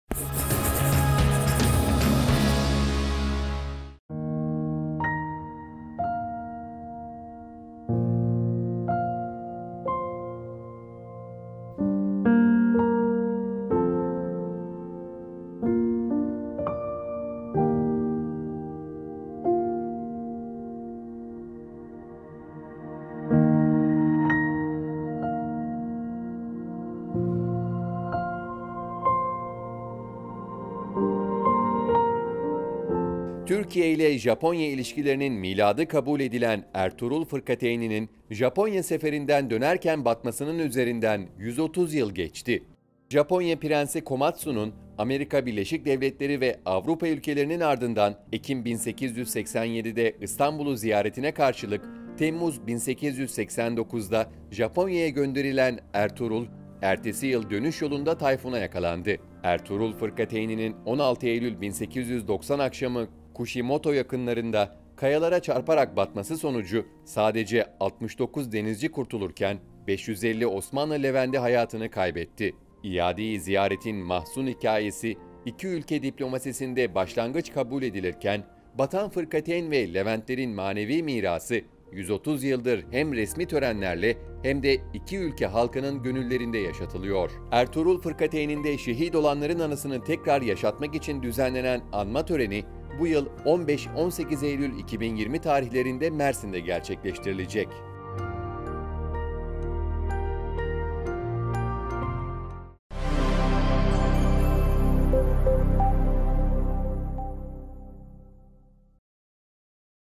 Ertuğrul Fırkateyni'nin 130. yılı anması Mersin'de yapılacak
Japonya’nın Kushimoto kenti açıklarında 1890’da kayalıklara çarparak batan Ertuğrul Fırkateyni’nde hayatını kaybeden 587 Türk denizci, şehit oluşlarının 130. yılında Mersin’de düzenlenecek törenle anılacak.